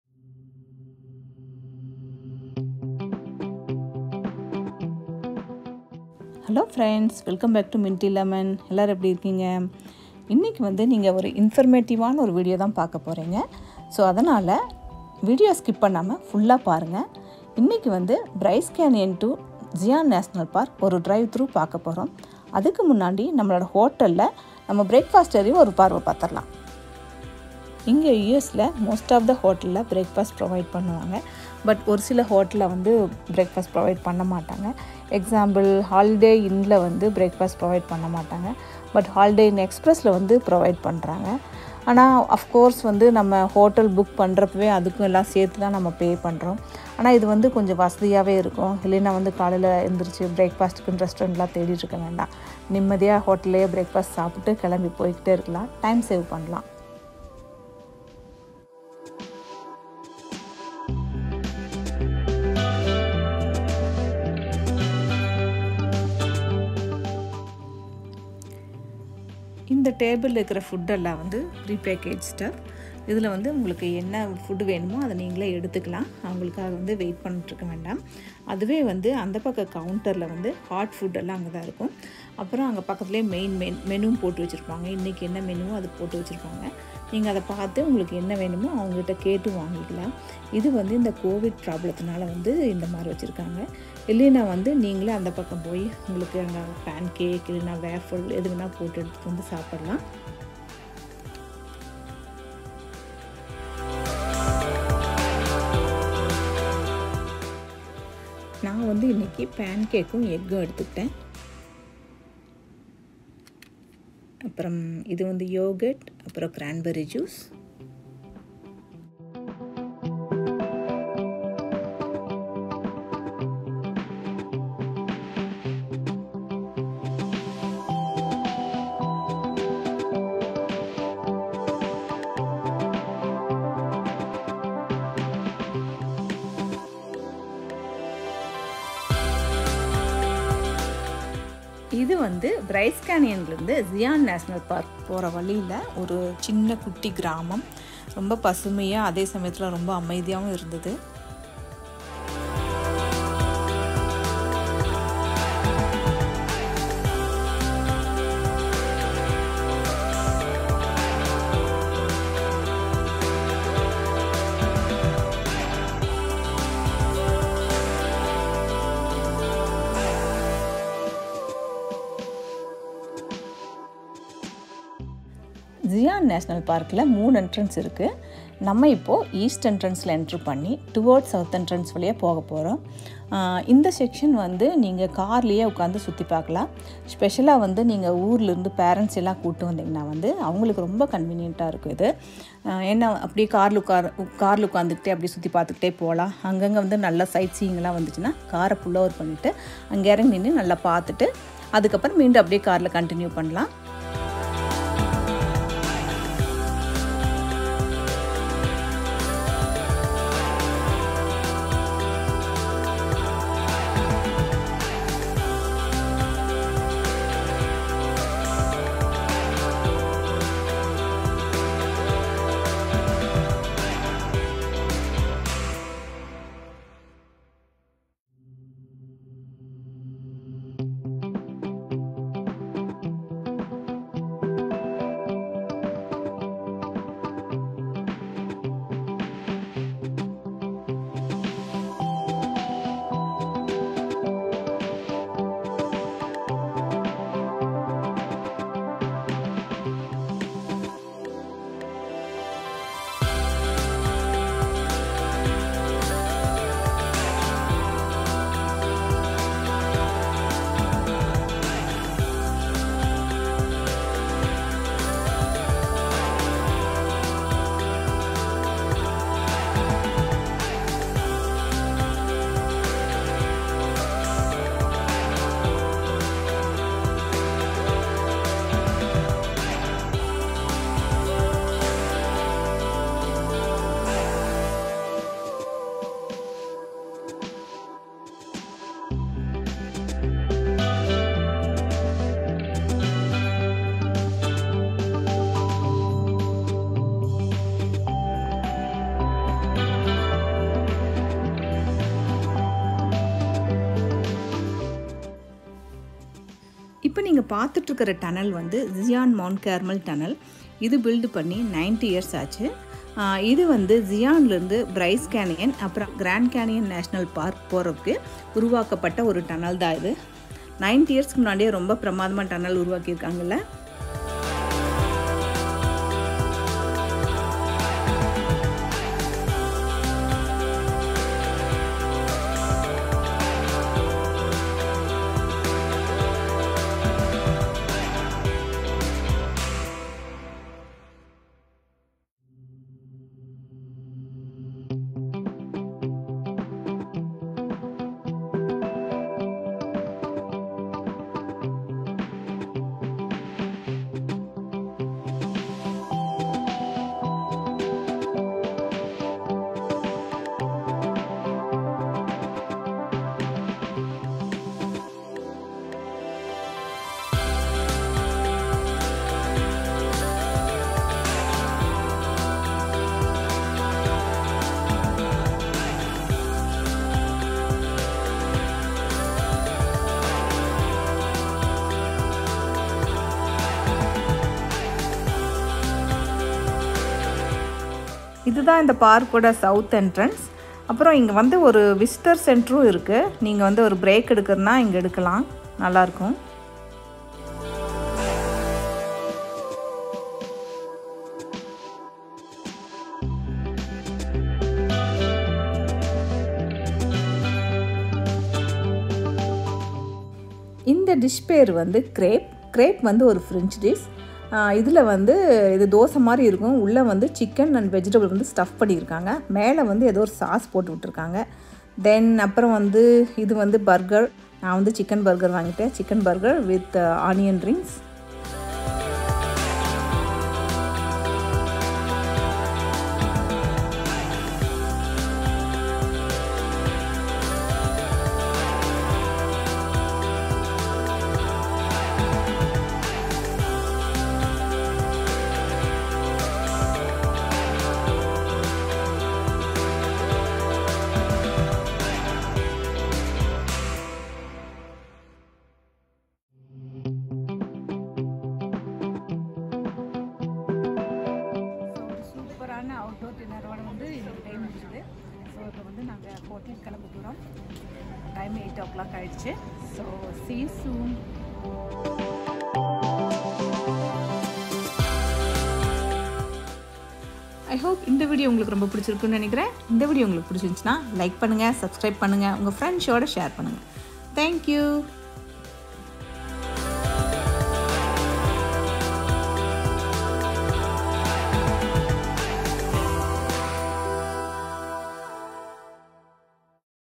Minty Lemon - Travel | Zion National Park - Scenic Drive | US Travel Tamil Vlog | English Subtitles
hello friends, today we are going to see a drive through at Zion National Park!